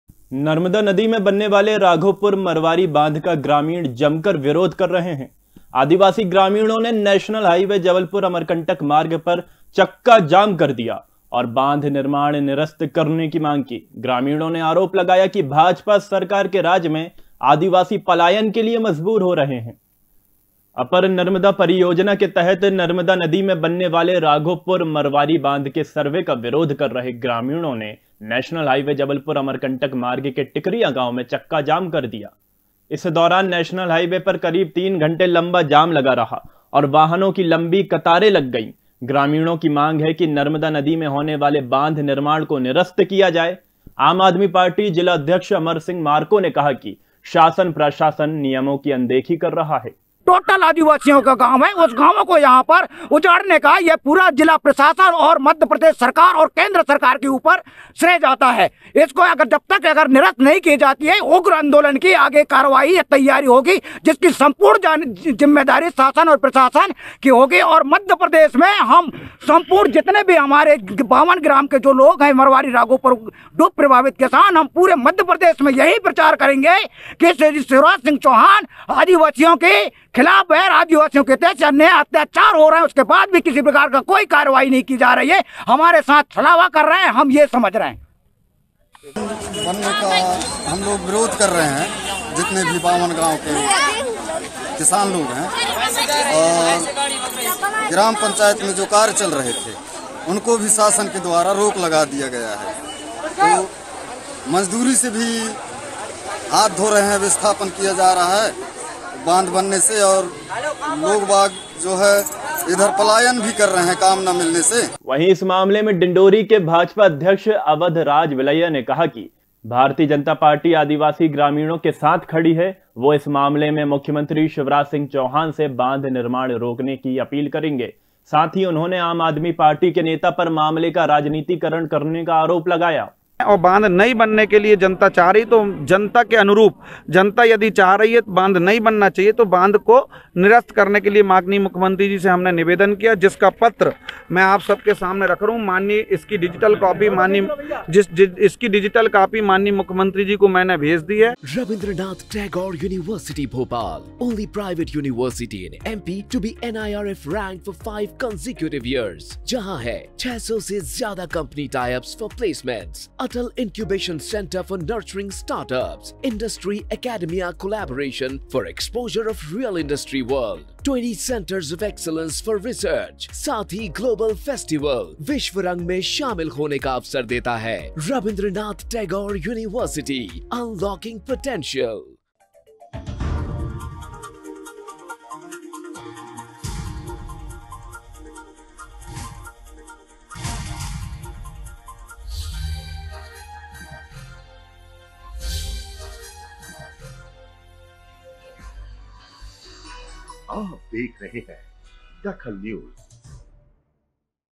DAKHAL NEWS आदिवासी ग्रामीणों ने किया चक्काजाम
आदिवासी ग्रामीणों ने किया चक्काजाम
नर्मदा नदी में बनने वाले बांध का विरोध